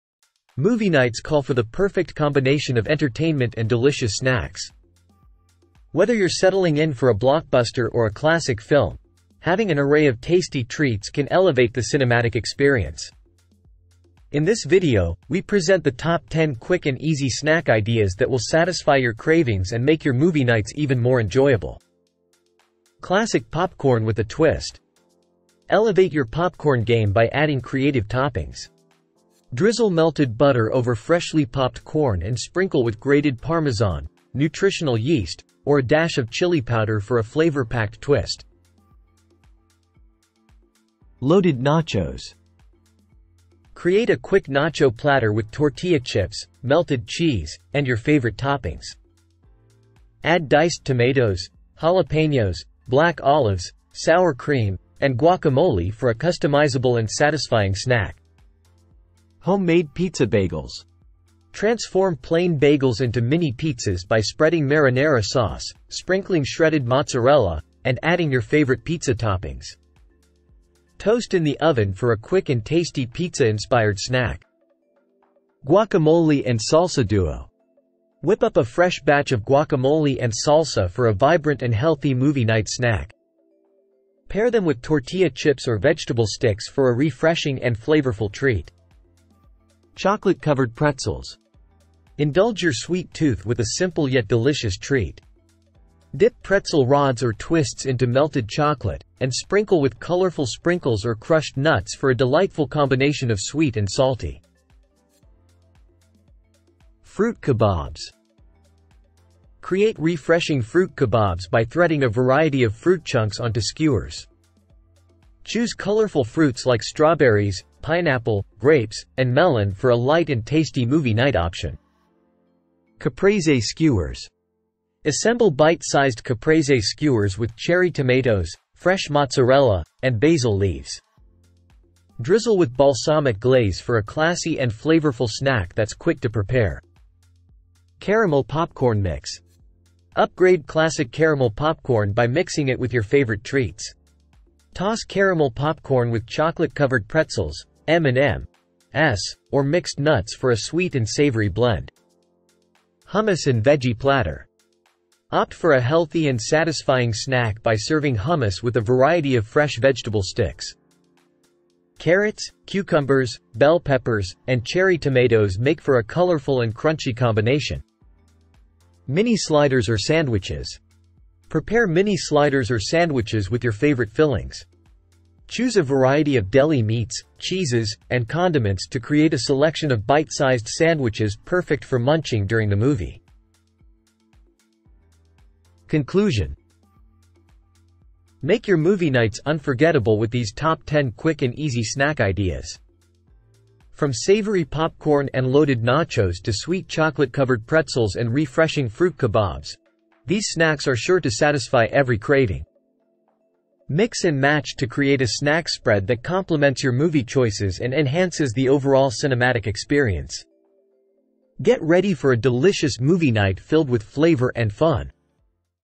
Top 10 Quick and Easy Snack Ideas for Movie Nights
Elevate your movie nights with these top 10 quick and easy snack ideas. From savory bites to sweet treats, discover snacks that make your cinematic experience even more enjoyable.